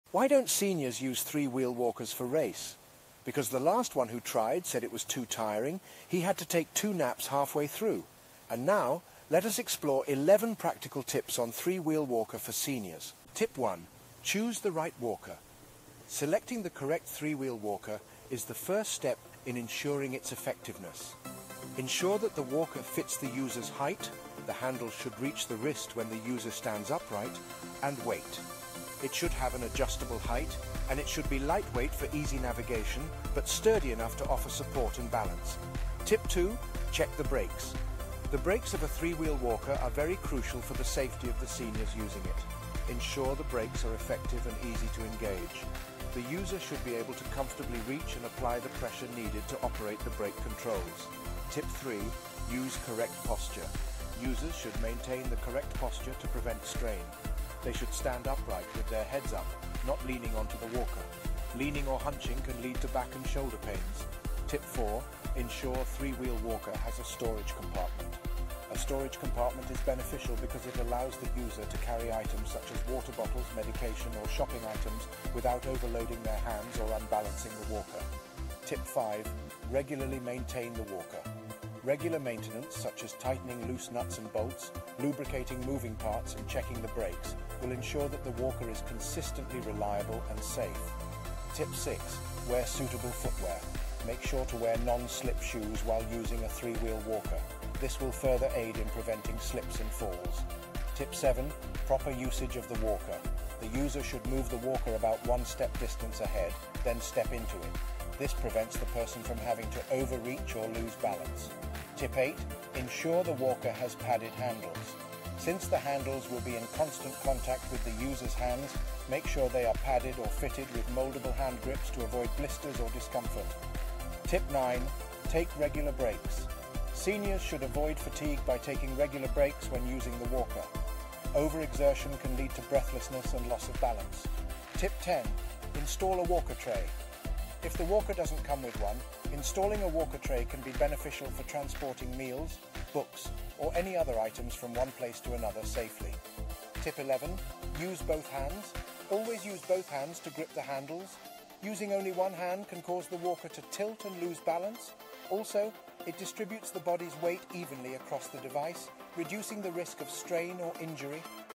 11 Tips On 3 Wheel Walker For Seniors
Looking for tips on using a three-wheel walker effectively and safely? This video offers eleven comprehensive tips - from selecting the right walker, checking the brakes, adopting the proper posture, to the proper usage of the walker, and more. Learn how to keep your device reliable with regular maintenance, utilize the storage compartments, and ensure your comfort with padded handles. Plus, find out why non-slip shoes and regular breaks are crucial, and how a walker tray can simplify your everyday life. A must-watch for seniors and their caregivers! Safety and convenience rolled into one comprehensive guide!